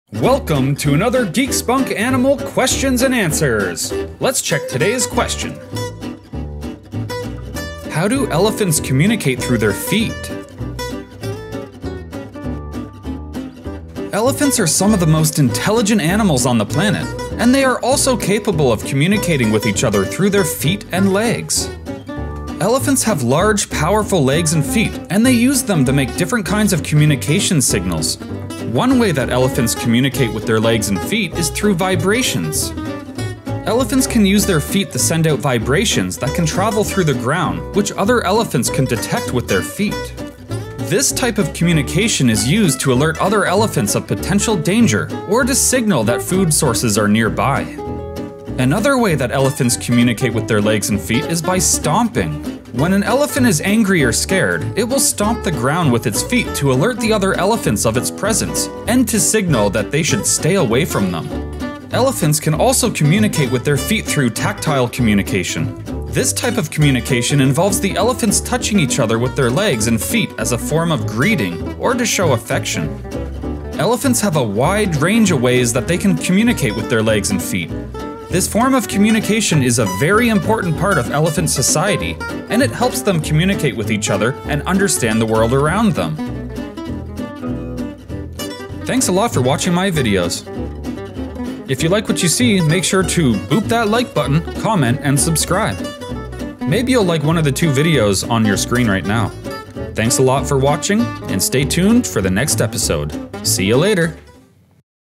How To Elephants Communicate Through Their Feet?
Elephants are some of the most intelligent animals on the planet, and they are also capable of communicating with each other through their feet and legs. Elephants have large, powerful legs and feet, and they use them to make different kinds of communication signals.

One way that elephants communicate with their legs and feet is through vibrations. Elephants can use their feet to send out vibrations that can travel through the ground, which other elephants can detect with their feet. This type of communication is used to alert other elephants of potential danger or to signal that food sources are nearby.

Another way that elephants communicate with their legs and feet is by stomping. When an elephant is angry or scared, it will stomp the ground with its feet to alert the other elephants of its presence and to signal that they should stay away.

Finally, elephants can also communicate with their feet through tactile communication. This type of communication involves the elephants touching each other with their legs and feet as a form of greeting or to show affection.

Overall, elephants have a wide range of ways that they can communicate using their legs and feet. This form of communication is an important part of elephant society, and it helps them communicate with each other and understand the world around them.